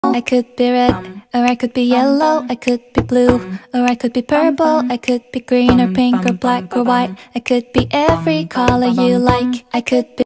Mixing Colors ~ Watercolor Drops
Using a watercolor brush pen to color the water drops to the beat of the song. Hope you enjoy this! 😃